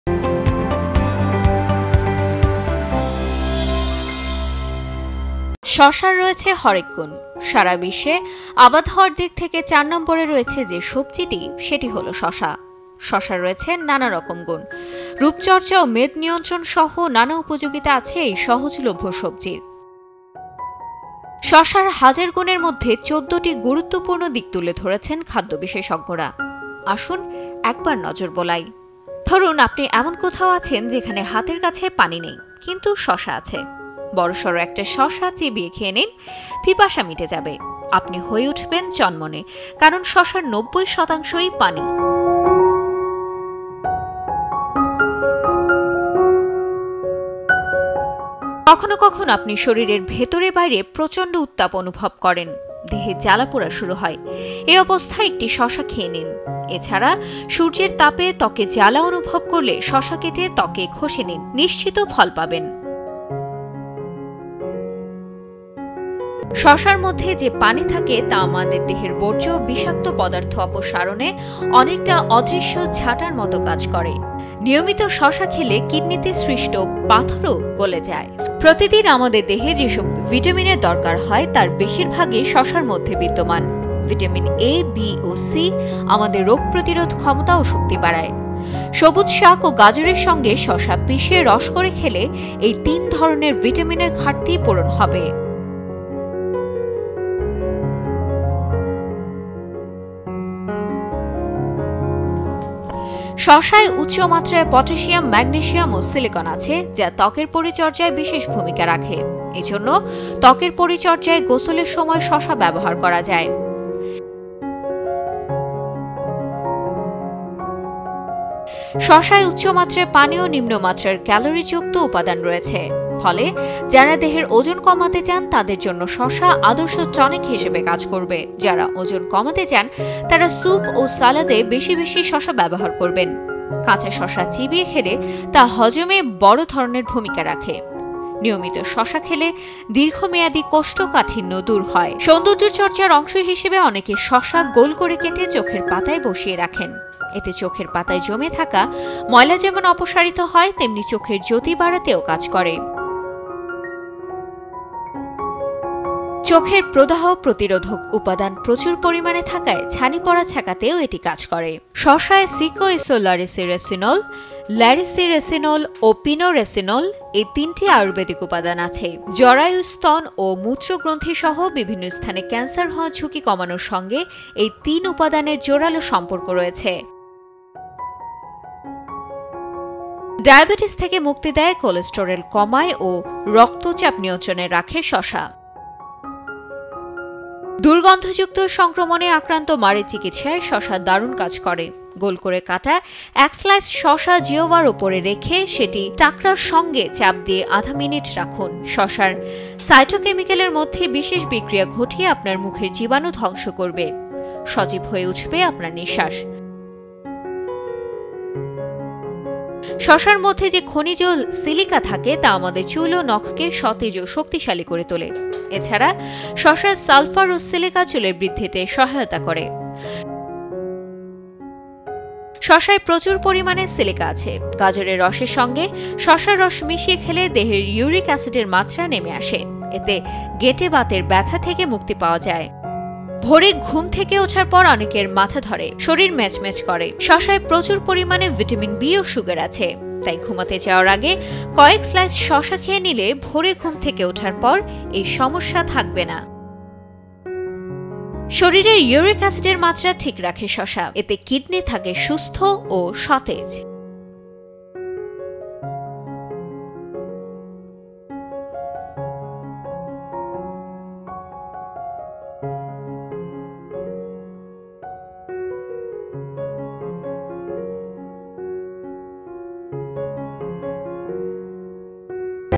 প্রতিদিন শসা খাওয়ার ১৪ টি উপকারিতা জেনে নিন cucumber eating benefits Health Cafe
নিয়মিত শসা খাওয়ায় কিডনিতে সৃষ্ট পাথরও গলে যায়। প্রাত্যহিক ভিটামিনের শূন্যতা পূরণ করে প্রতিদিন আমাদের দেহে যেসব ভিটামিনের দরকার হয়, তার বেশির ভাগই শসার মধ্যে।